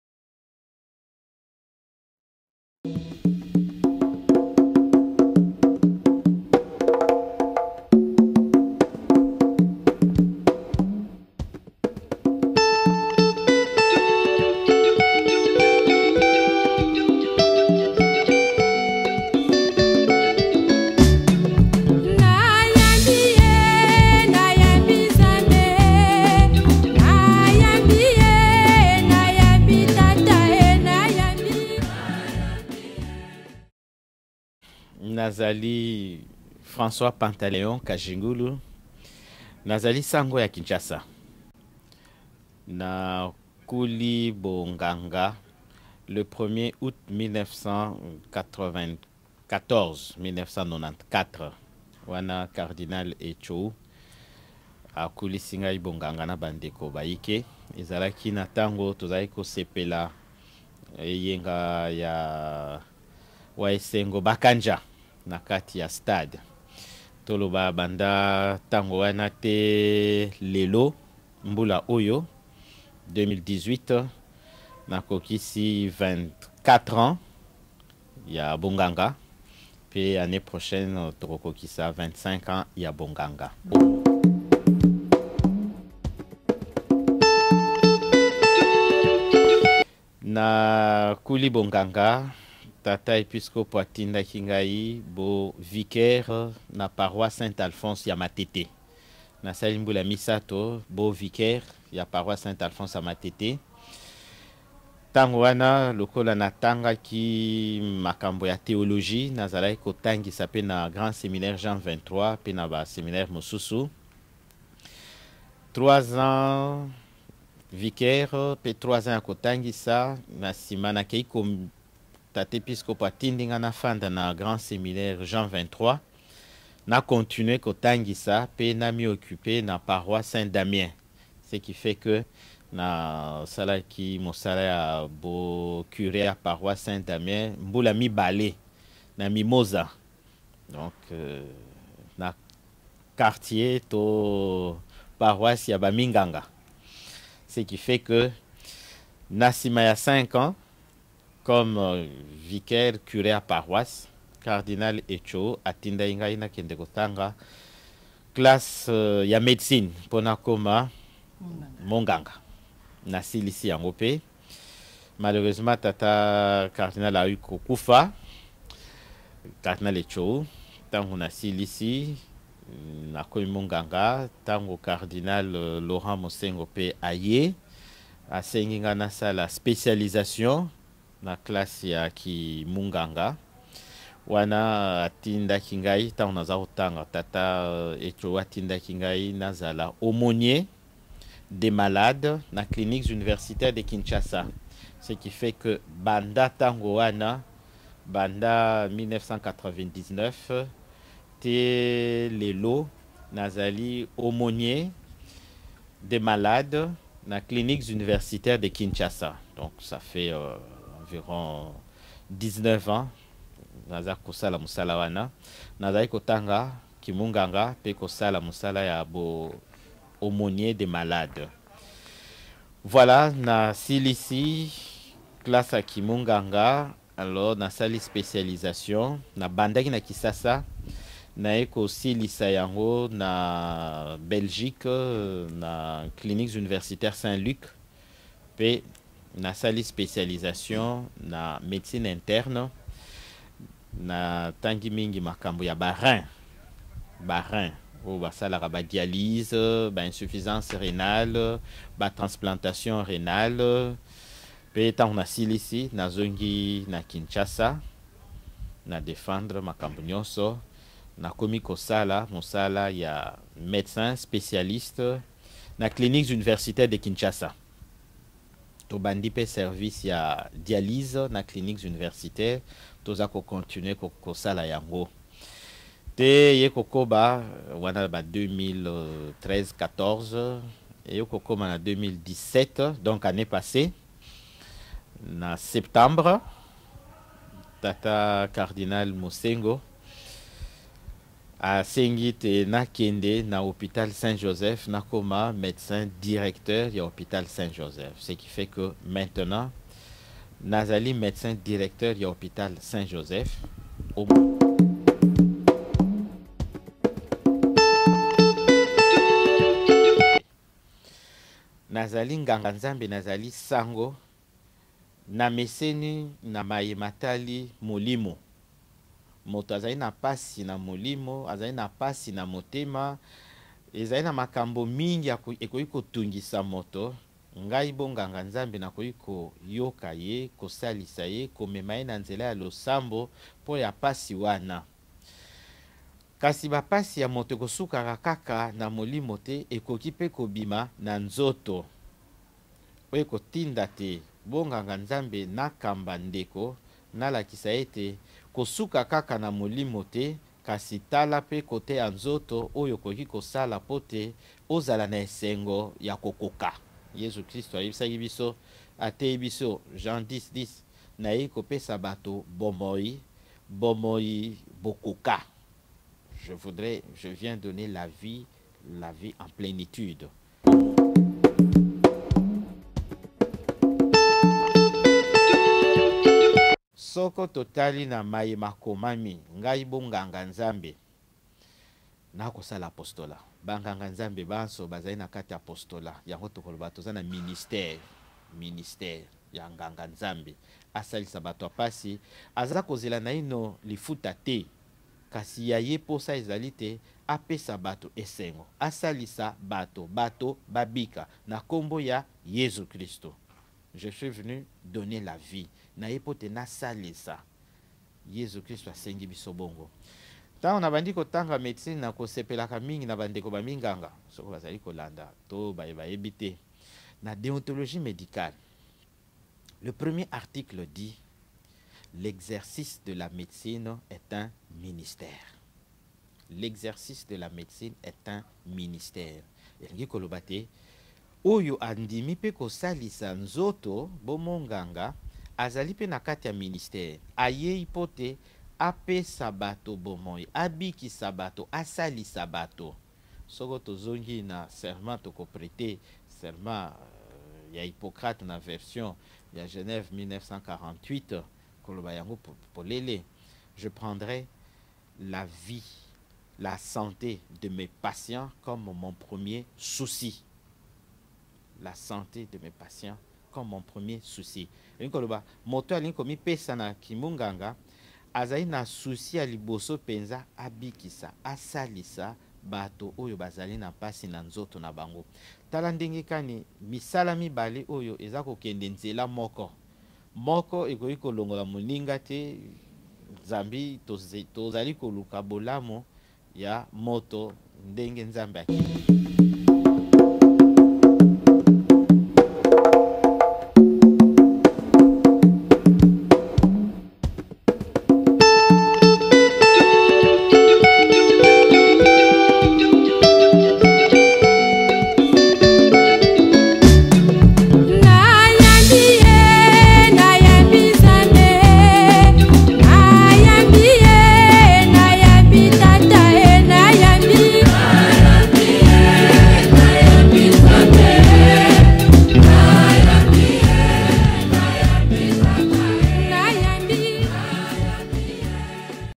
NAYAMBI / Le Portrait de Abbé François Pantaleon KAJINGULU
Le portrait l Abbé François Pantaleon KAJINGULU médecin directeur de l'hôpital Catholique St Joseph de Kinshasa